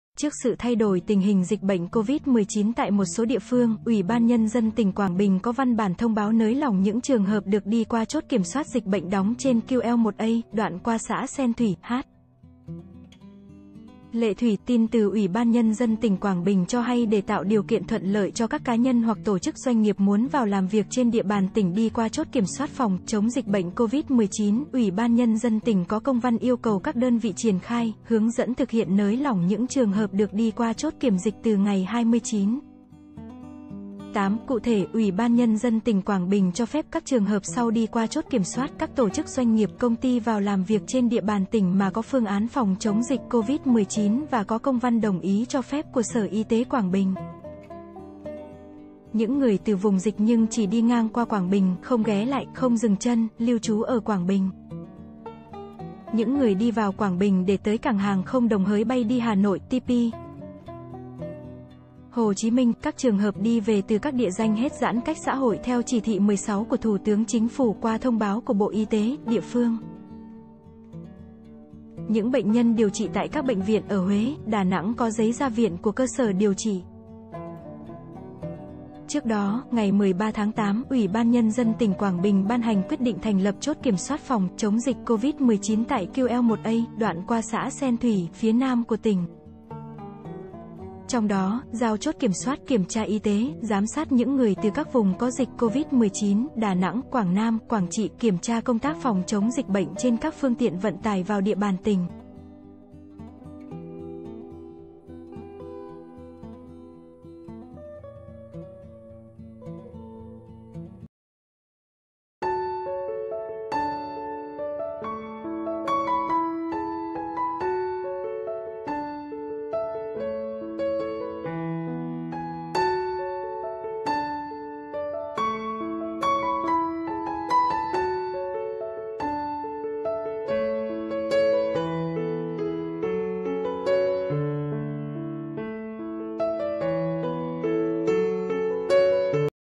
Trường hợp nào được đi qua chốt kiểm dịch Covid-19 ở Quảng Bình?
Trước sự thay đổi tình hình dịch bệnh Covid-19 tại một số địa phương, UBND tỉnh Quảng Bình có văn b.